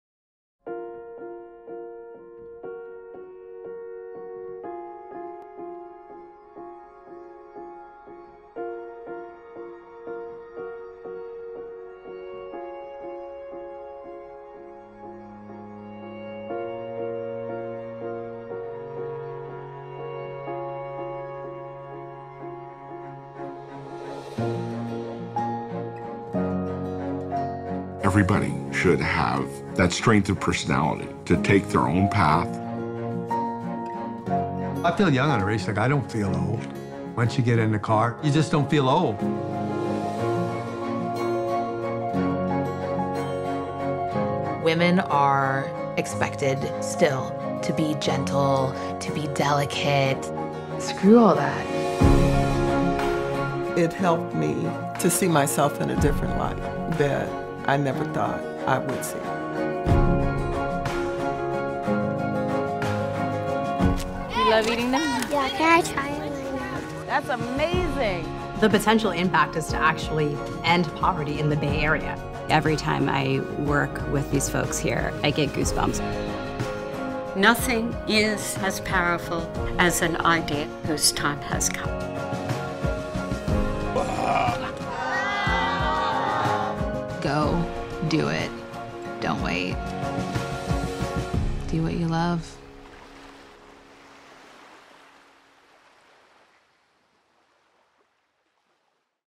Sub64Films Demo Reel
Demo Reel for Sub64Films

All work produced and directed by Sub64Films / Kevin Gordon with the following exceptions:
*Tipping Point Video Directed by Kevin Gordon, Produced by Even Odd Films
*JVS Videos, Directed by Kevin Gordon, Produced by 01 Productions